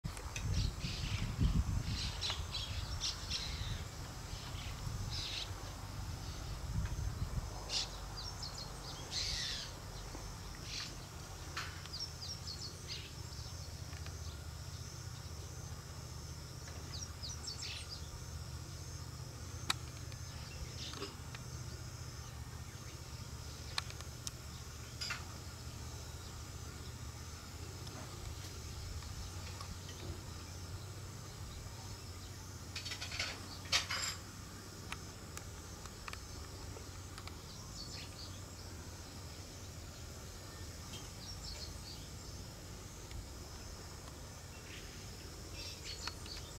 つくね芋の生長 キュウリネットの一番上まで伸びた
つくね芋をHDで撮影してみました。